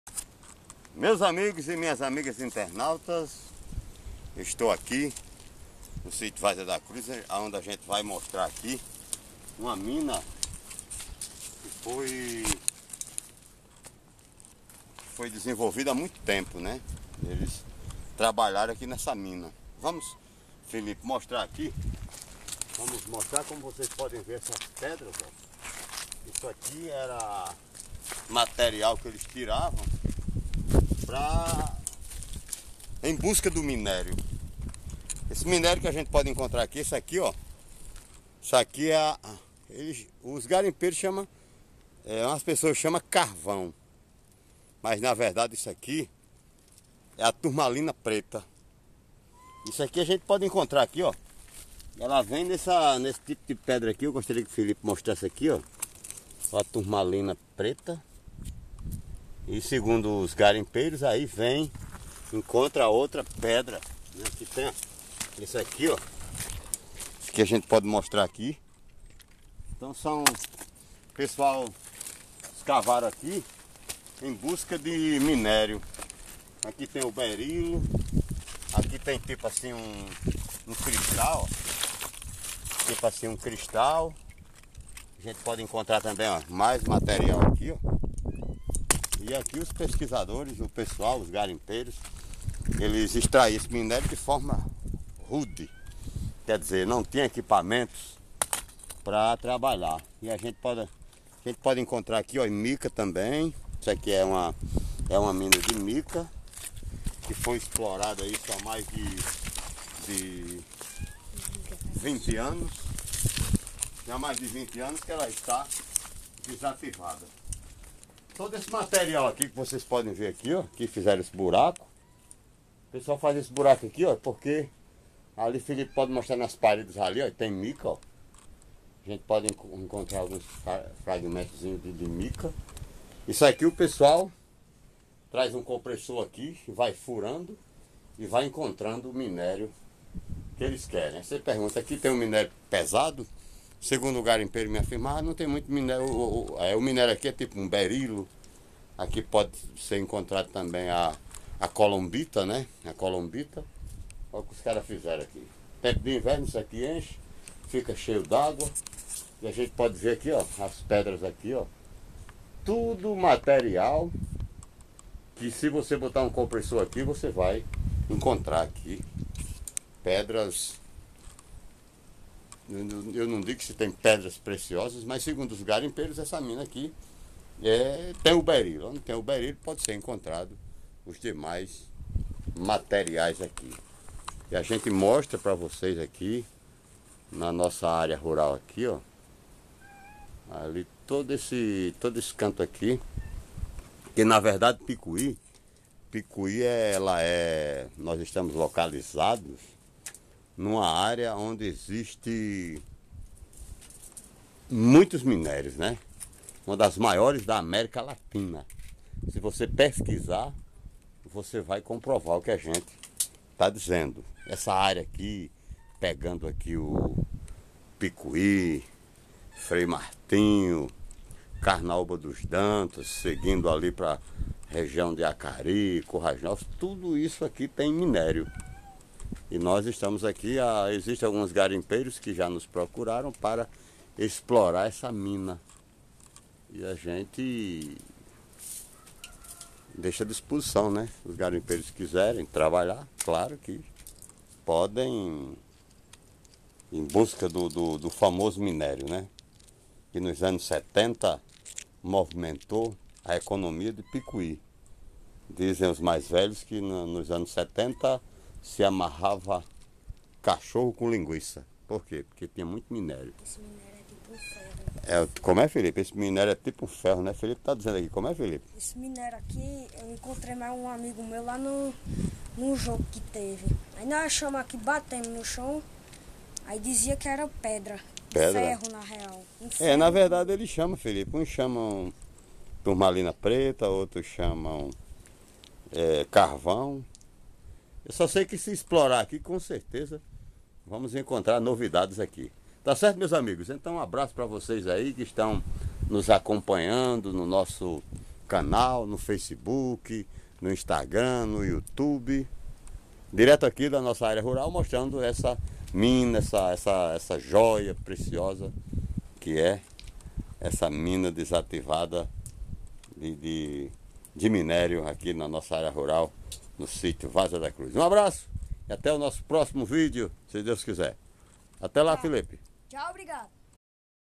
Visitando minas em Picuí PB.
Geraldo Batista faz una visita a uma mina que está desativada no sítio Várzea da Cruz no município de Picuí. veja a riqueza que tem o nosso subsolo.